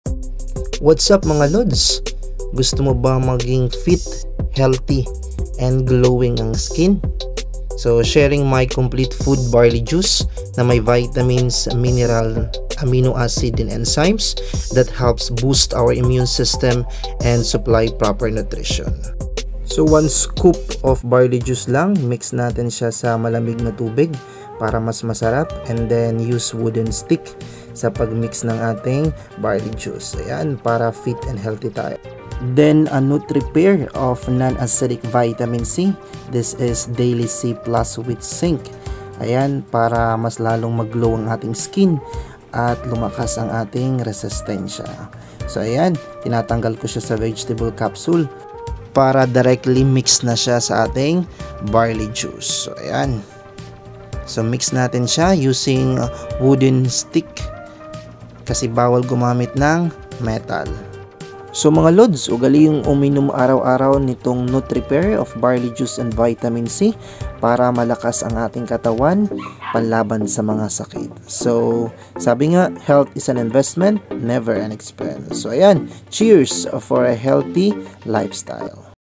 Gusto mo ba maging Fit, Healthy body and Glowing Skin? Uminom ng Barley Juice araw-araw
SANTE BARLEY is the only vegetation on earth that can supply nutritional support from birth to old age. Nutripair barley juice and Vitamin C